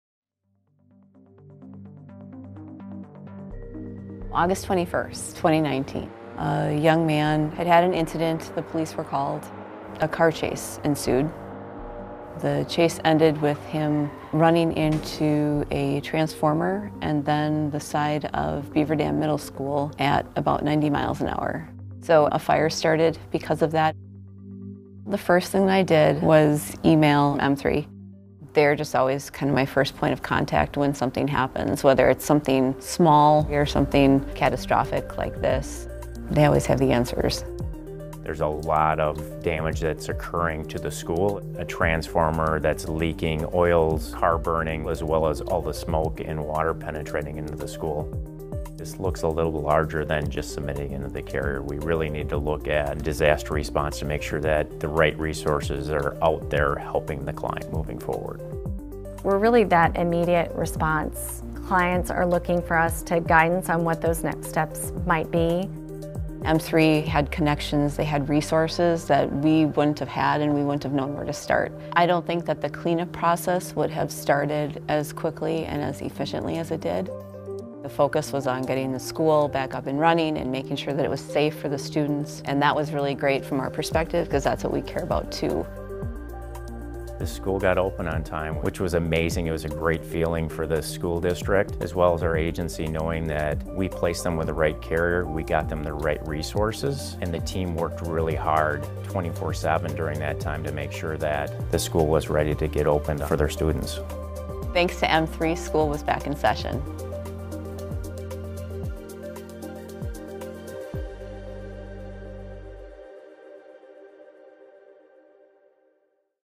Beaver Dam School District - An M3 Rapid Response & Recovery (R3) Story
When the unthinkable happened, Beaver Dam School District relied on their partnership with M3 and its Rapid Response & Recovery (R3) team. Soon, school was back in session.